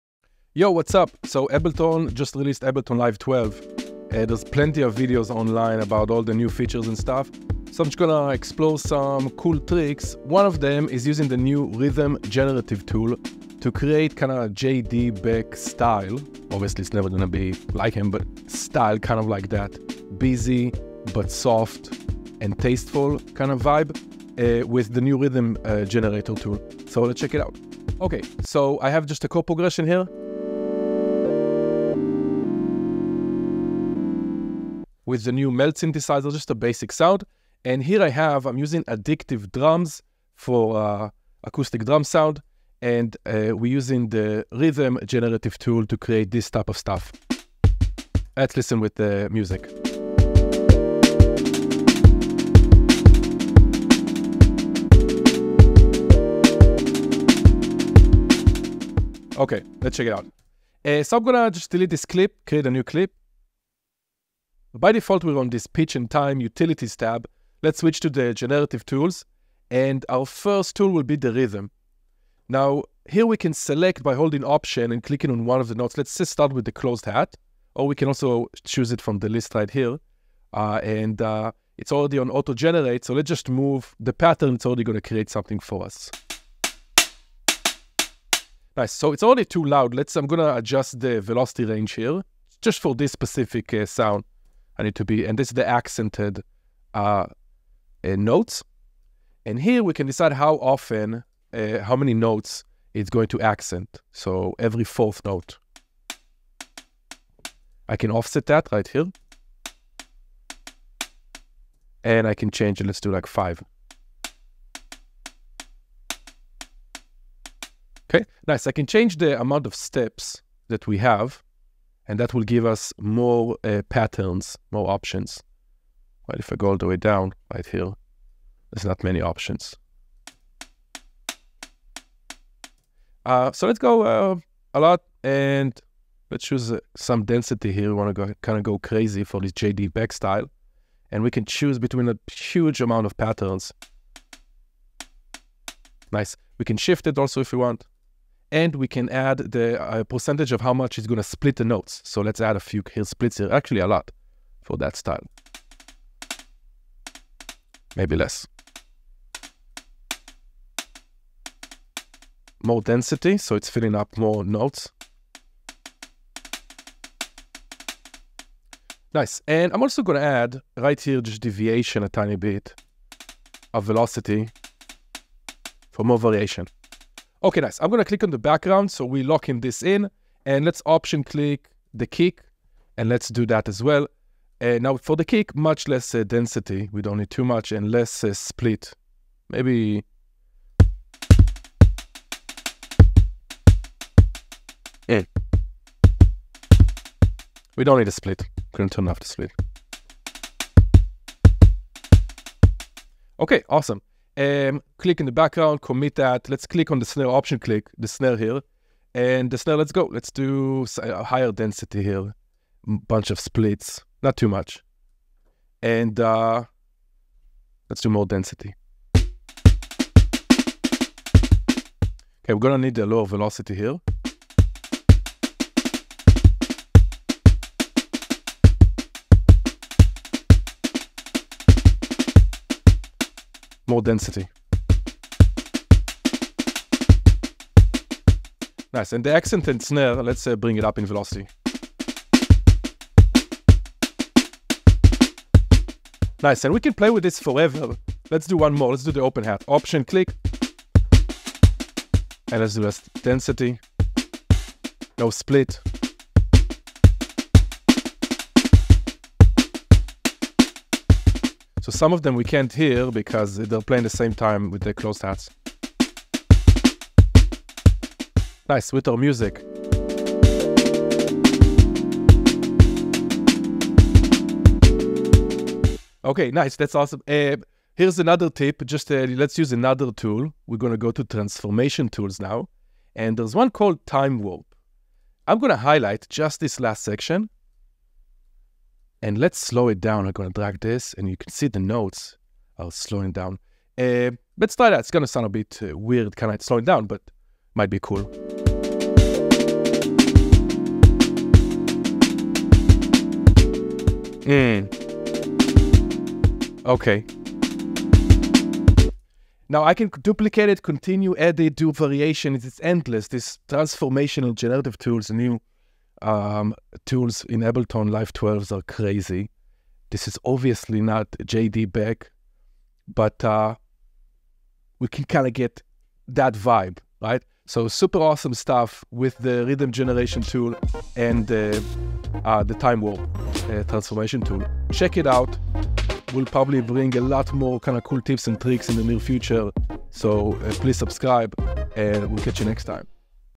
Programming JD Beck Style Drums with Ableton Live 12 New Rhythm Generative Tool | Side Brain
This new Ableton Live 12 generative tools are crazy!

* Check out the Freebies section!

@sidebrain on socials

Ableton Certified Trainer

0:00 - Intro
0:28 - Rhythm Generative Tool
4:36 - Time Warp
5:10 - Outro